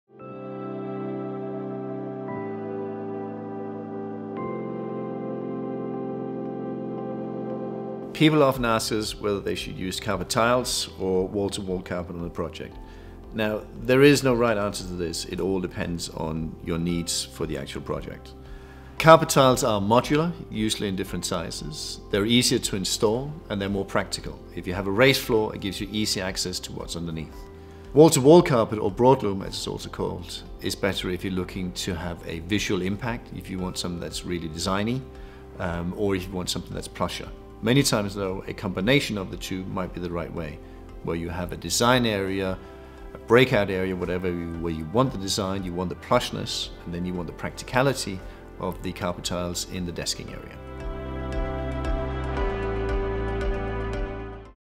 Should I use carpet tiles or wall to wall carpets?
ege Global Account Manager Kim Monrad answers our most frequently asked questions about carpets. In this video, Kim explains how to choose between carpet tiles and wall to wall carpets (broadloom) based on your needs and your project.